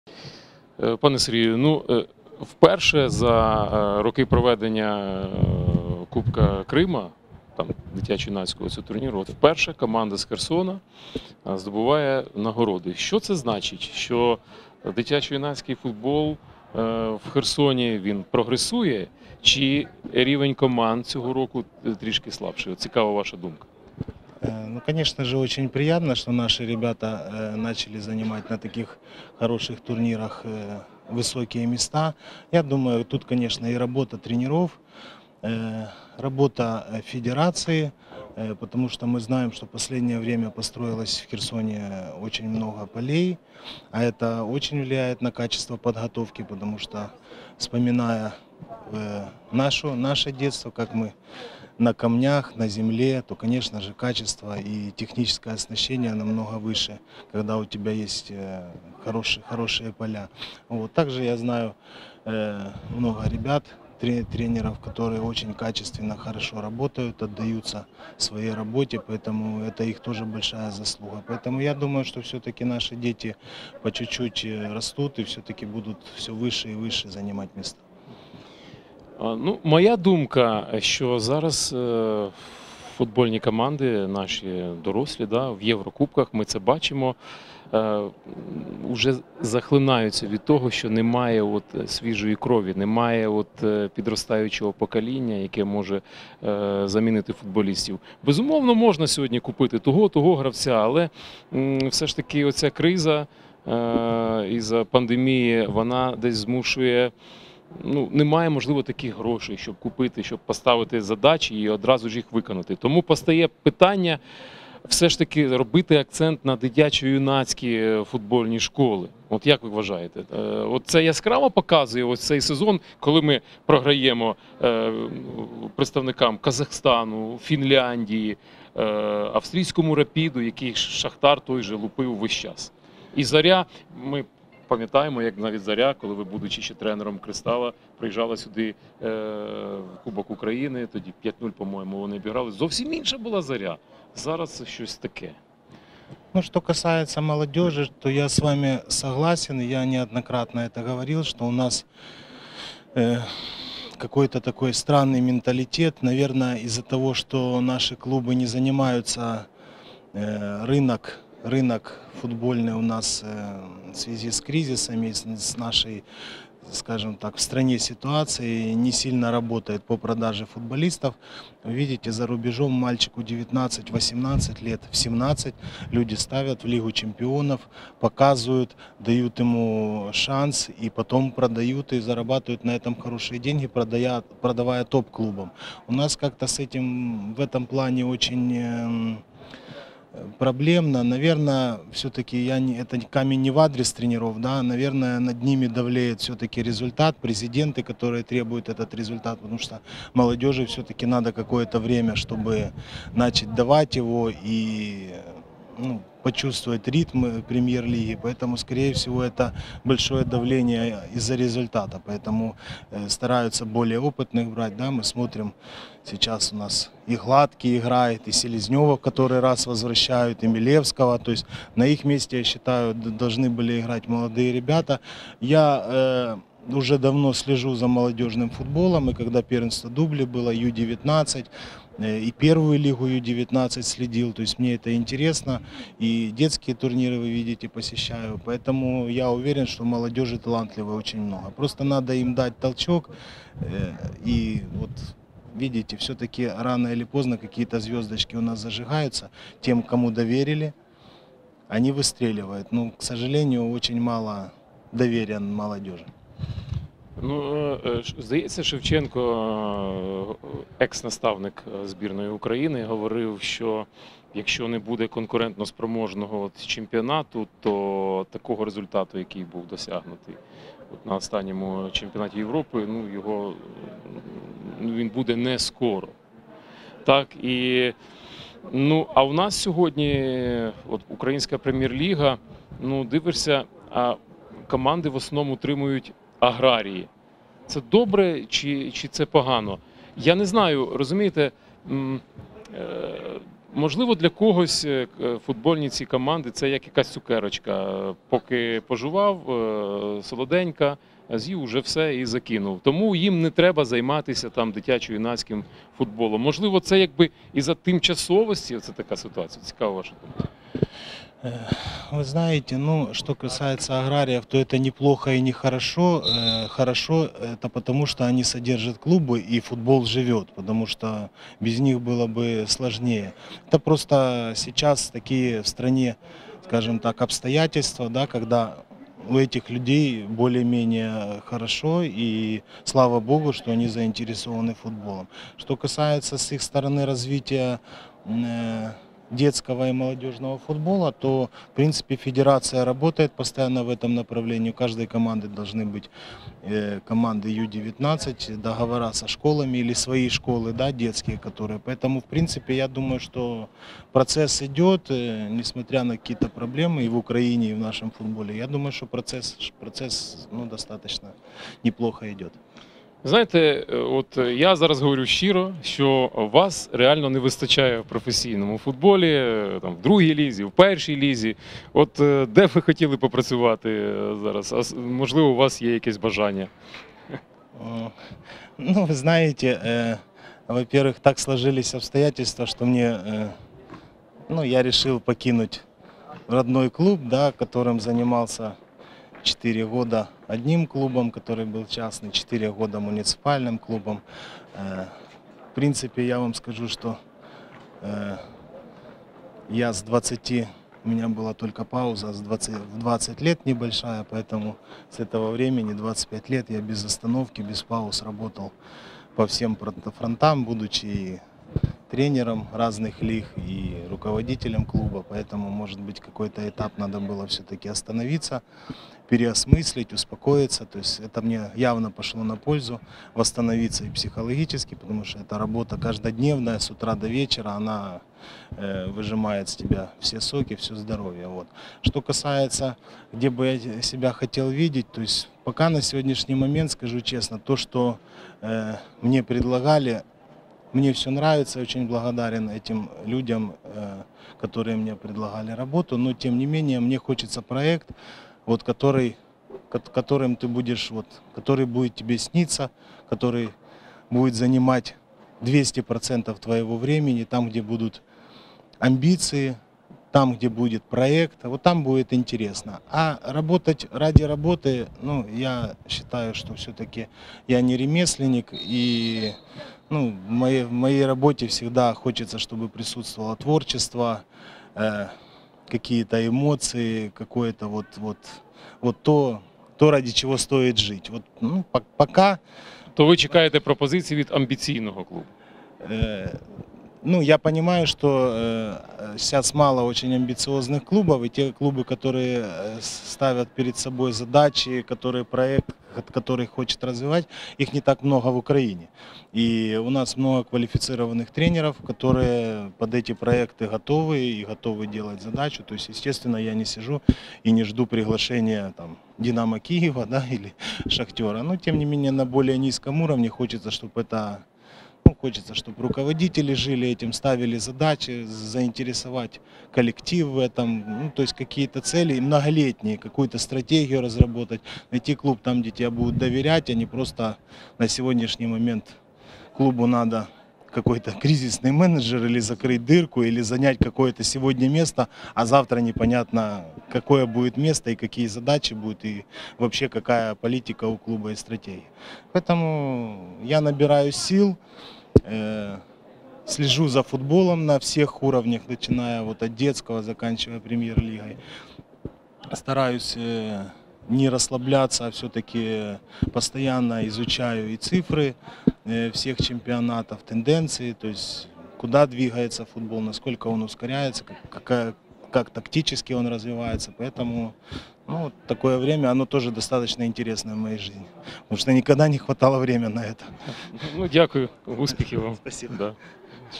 Інтерв'ю: Сергій Шевцов - екс-тренер МФК "Кристал" Херсон.
Інтерв'ю: Сергій Шевцов - екс-тренер МФК "Кристал" Херсон. VI традиційний турнір «Крим — це Україна»